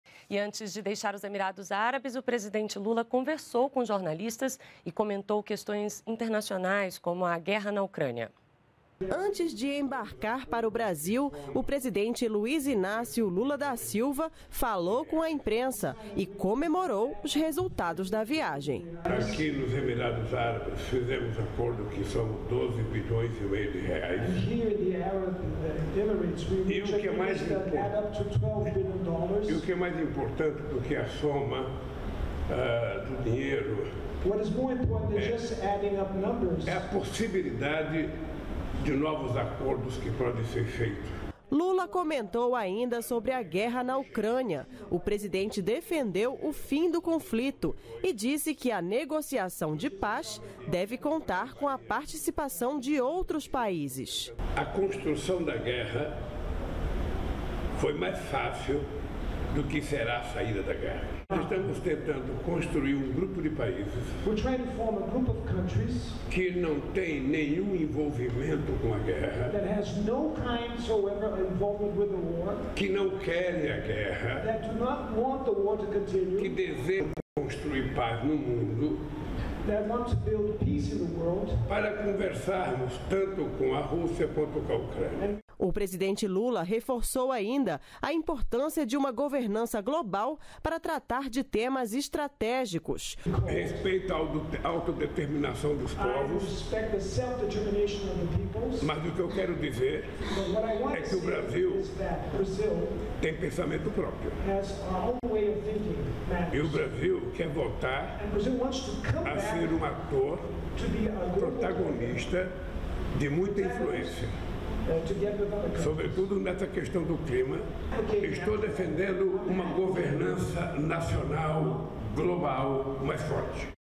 Relações Exteriores | Guerra na Ucrânia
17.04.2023 - Antes de deixar os Emirados Árabes, o presidente Lula conversou com jornalistas e comentou questões internacionais, como a Guerra na Ucrânia.